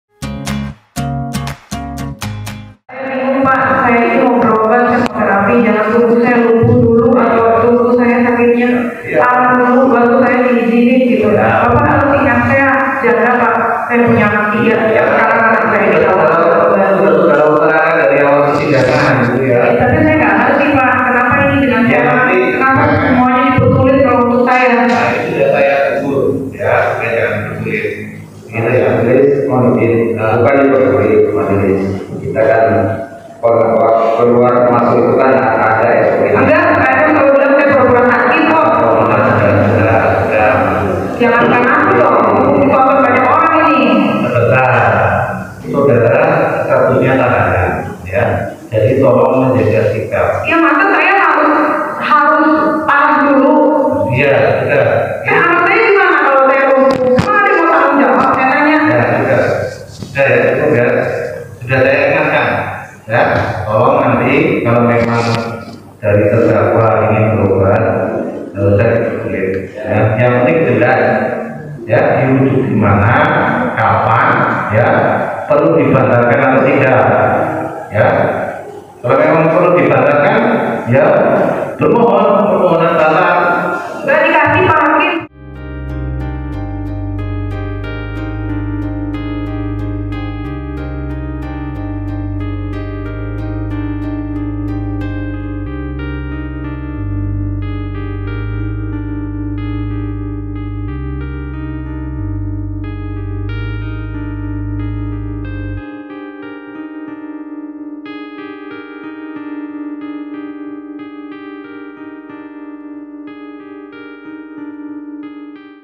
Nikita Mirzani Banting Mic dan Lempar Berkas Pemeriksaan Kesehatan
IntipSeleb - Terdakwa kasus dugaan pencemaran nama baik, Nikita Mirzani kembali menjalani sidang di Pengadilan Negeri Serang pada Senin, 19 Desember 2022. Agenda sidang kali ini adalah pemeriksaan tiga orang saksi, salah satunya saksi korban, Dito Mahendra. Namun, Dito terlihat kembali tak hadiri sidang kali ini. (R-MI)